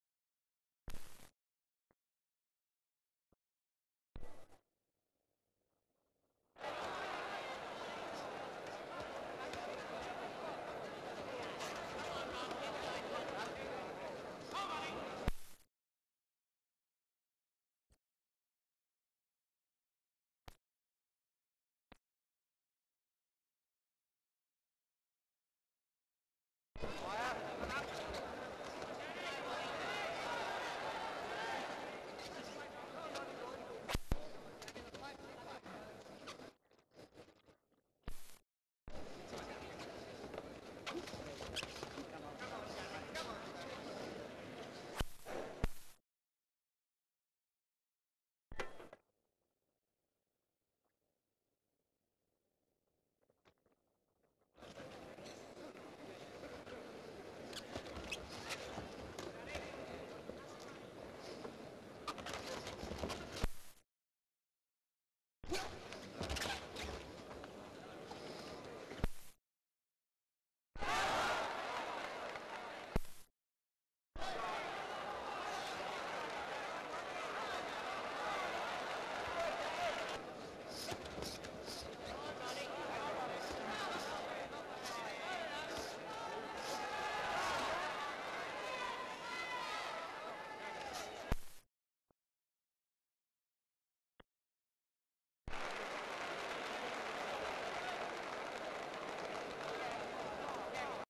Boxing: Ronnie Clayton V Danny O' Sullivan (1951)
Title - Boxing. Clayton v. O'Sullivan'.  Albert Hall, London.

Various shots of the fight in progress between Ronnie Clayton and Danny O'Sullivan, as people watch from ringside.

MS Clayton's hand raised in victory. LS crowd in Clayton's corner after fight, the two boxers embrace.

Used in AKC 250.
 FILM ID:2577.19